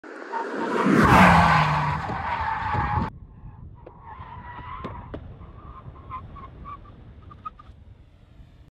BMW M8 Avoiding Obstacles At 150 km/h
When you travel at high speeds, braking is extremely important. This is why driving schools emphasize exercises where a car brakes and avoids obstacles from high speeds. In this example, the BMW M8 Coupe goes through such an exercise.

The test was performed during driving courses put together by @romaniadrivingexperience and executed by professional drivers on a track.